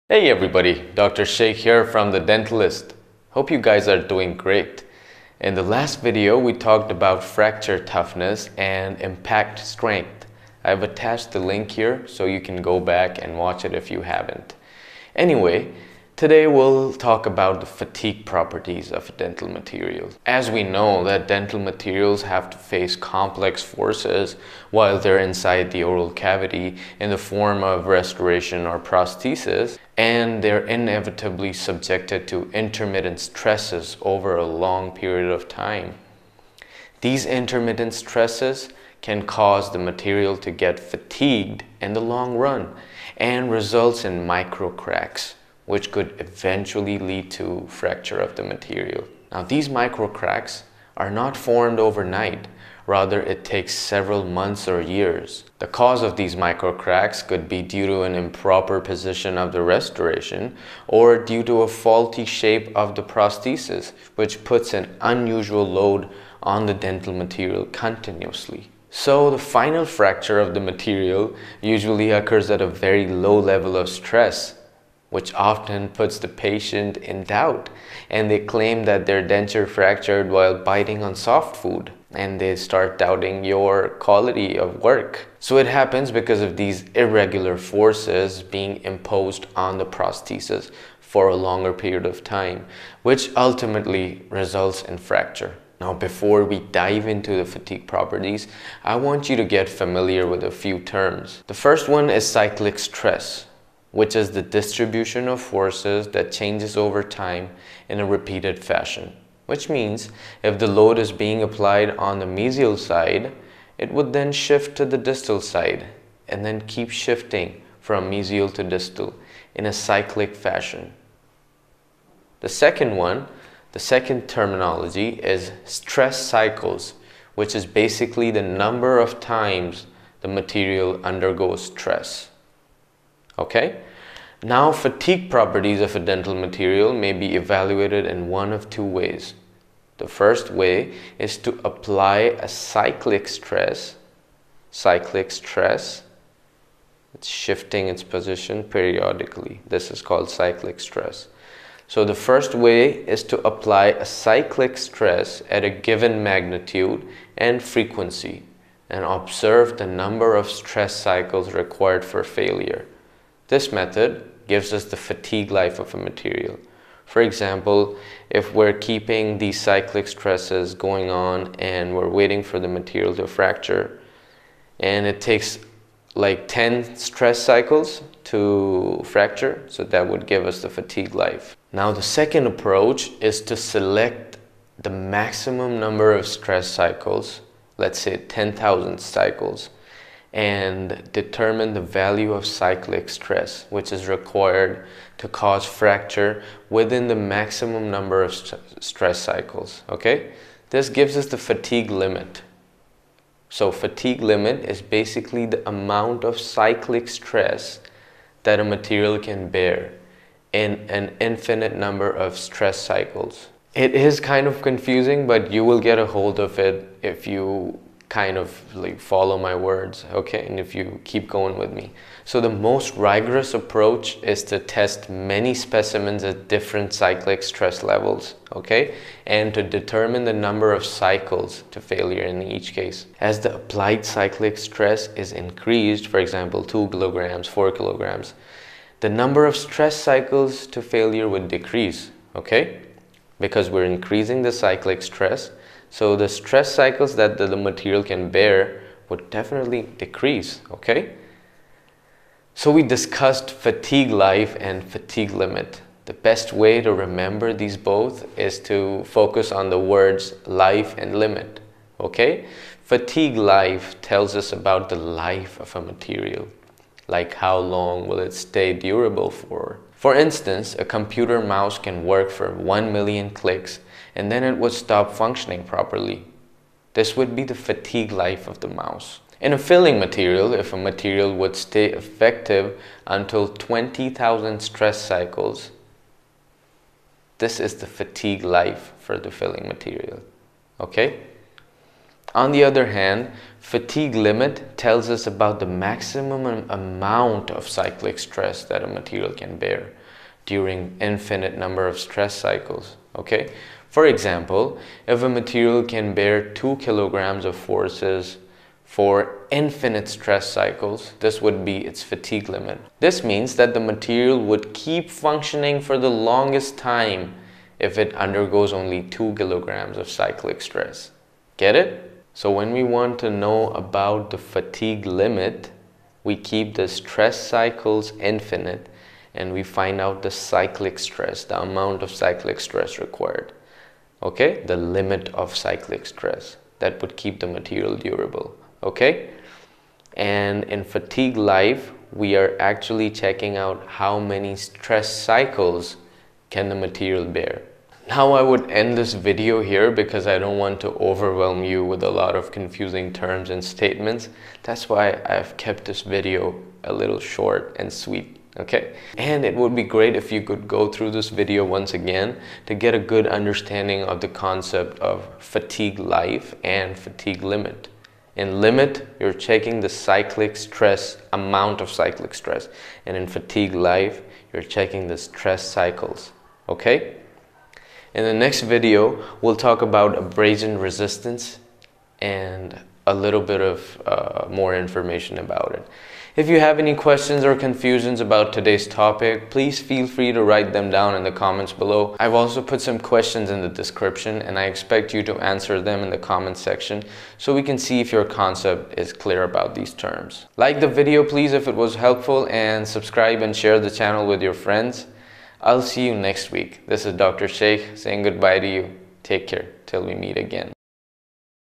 Fatigue Properties of Dental Materials
💬 *Book a free call* if you have any questions before joining

In this video Dr. Mujtaba Shaikh talks about the Fatigue properties of the dental materials, including the important concepts and terminologies like Cyclic Stress, Stress Cycles, Fatigue Life and Fatigue Limit.

Buy your textbook of Applied Dental Materials today, if you still haven't.

Quiz:

1. If a filling material is to be tested for its fatigue limit, what would we actually determine?
(a) amount of cyclic stress
(b) number of stress cycles
(d) amount of stress cycles

2. Stress cycles is the:
(a) number of cyclic stress
(b) amount of cyclic stress
(d) value of load

3. Fatigue life is determined by:
(a) (a) amount of cyclic stress

The book followed for all the lectures of Dental Materials is, Applied Dental Materials Science by Mc Cabe.

We are currently on Chapter 2, which is the Properties Used to Characterize Dental Materials.